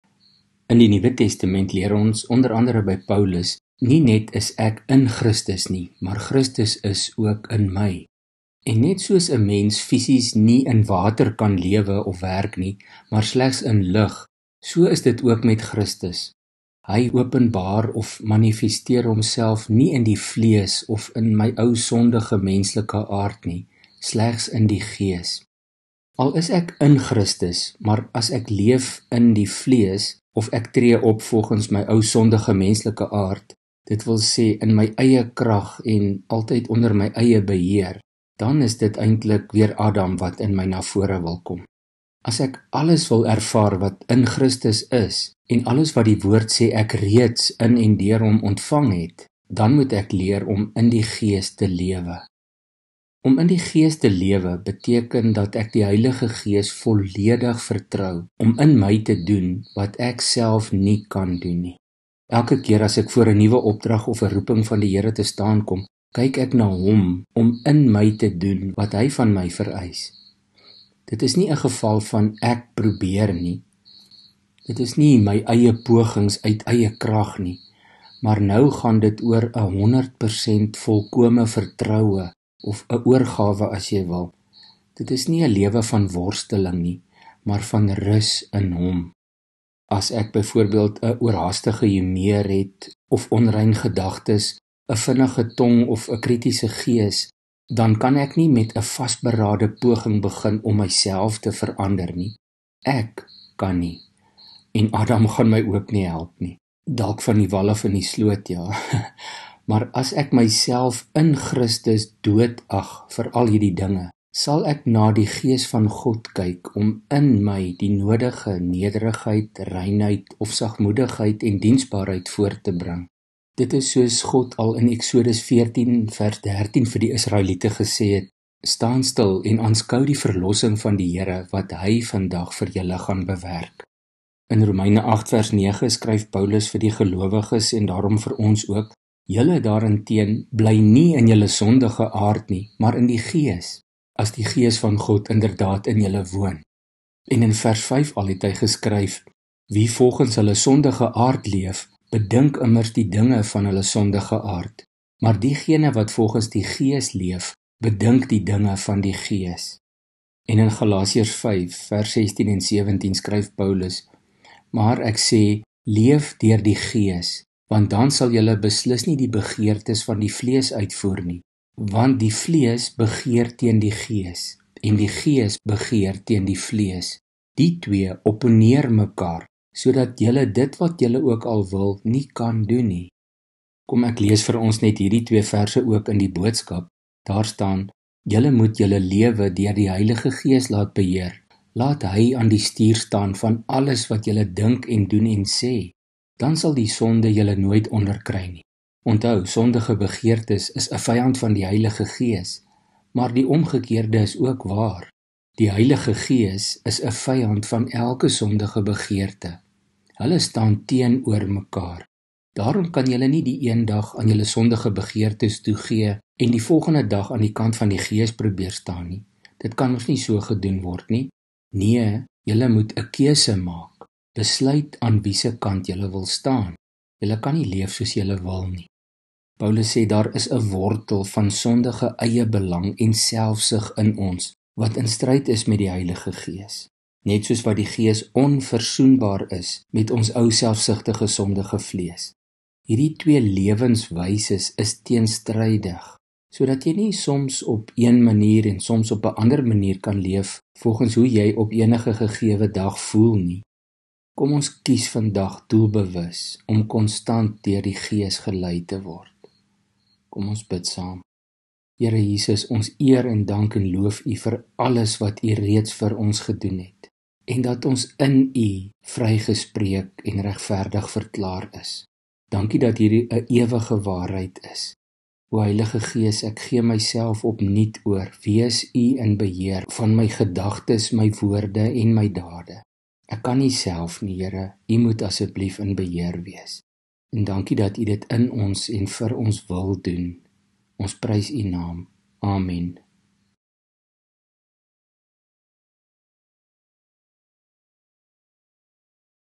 Leef deur die Gees - oordenking deur johannes vd merwe (ng kerk alkmaar)
Leef deur die Gees - oordenking deur johannes vd merwe (ng kerk alkmaar) - Rom 8:5, 9; Gal 5:16-17; Eks 14:13